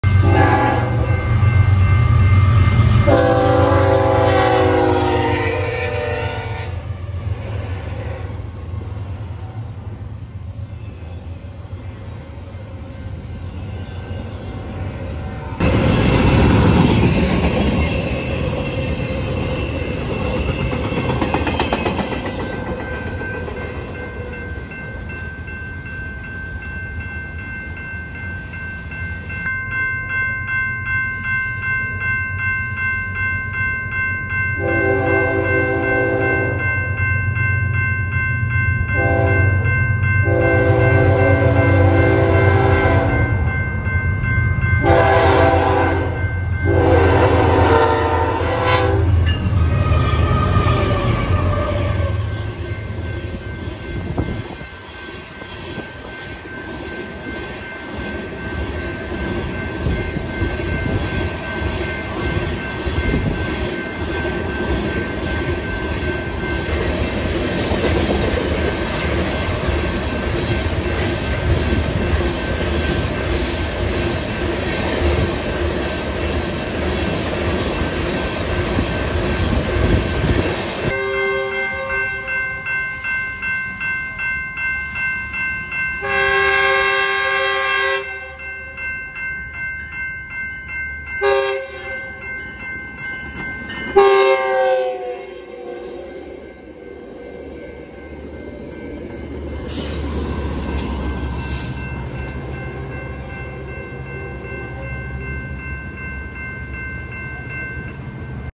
Santa Fe Springs Extras 10/28/2023
some extra train clips from santa fe springs!

these clips will not be in the halloween special on tuesday